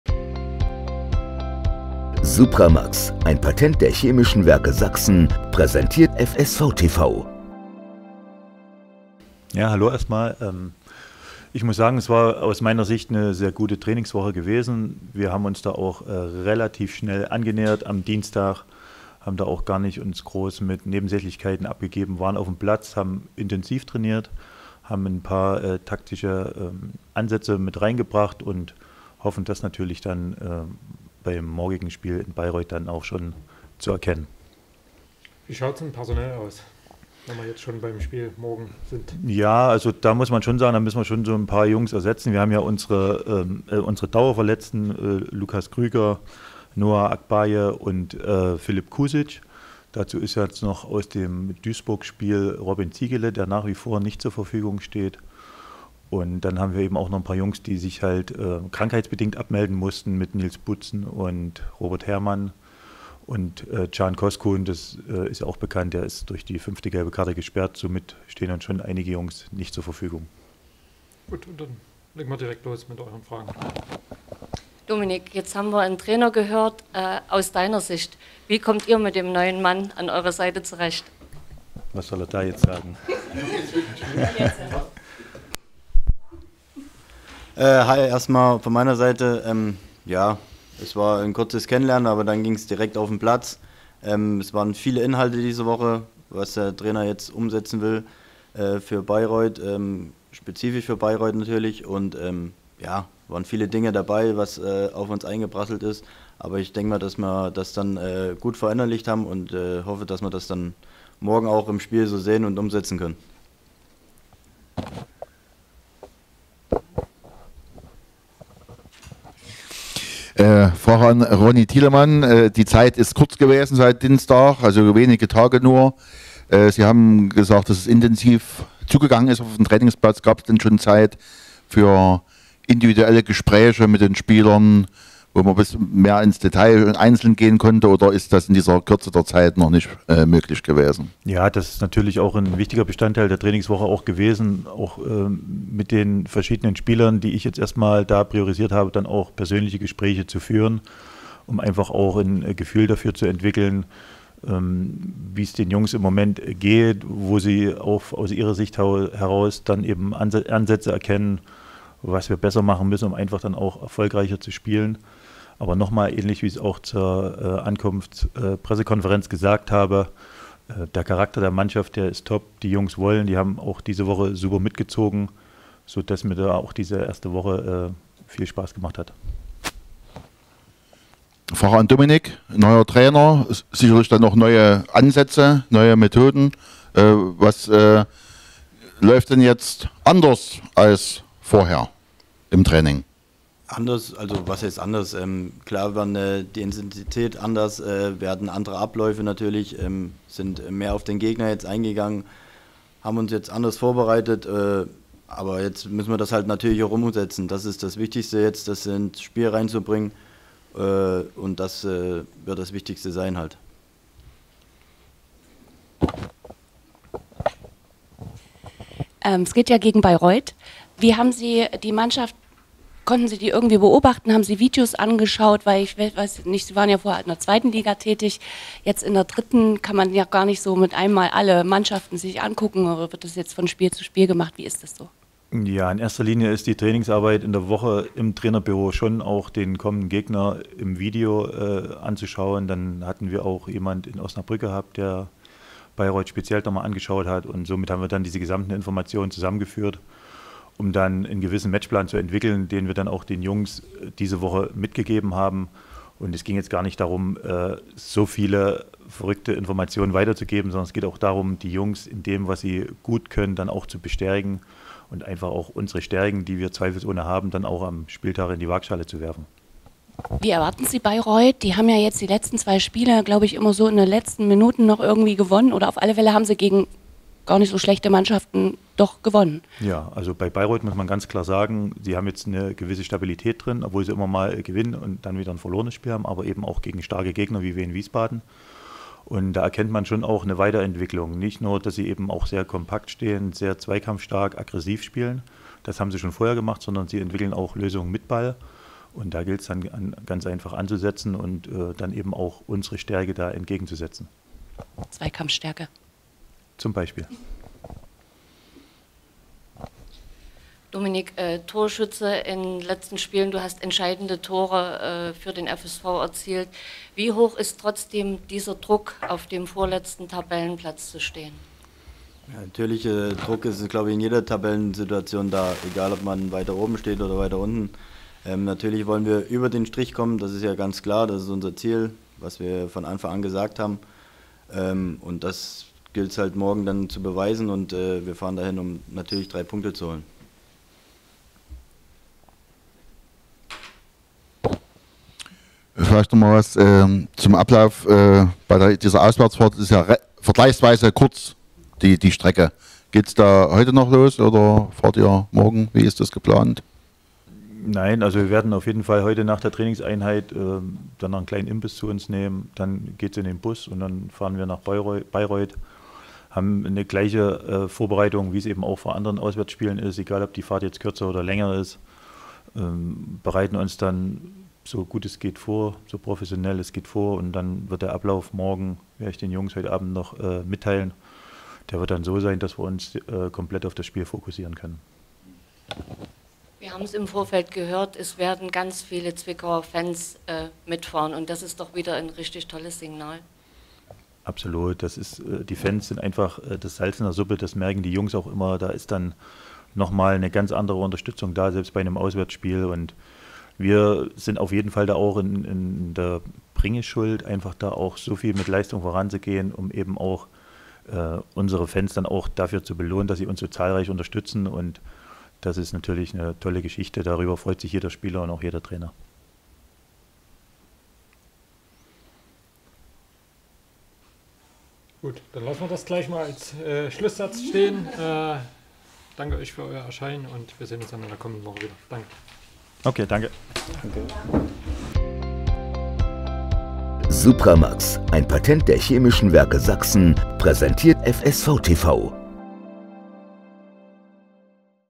FSV TV | AUSBLICK | Pressekonferenz vor dem Auswärtsspiel bei der SpVgg Bayreuth
Die Pressekonferenz mit Ronny Thielemann und Dominic Baumann vor dem Auswärtsspiel in Bayreuth.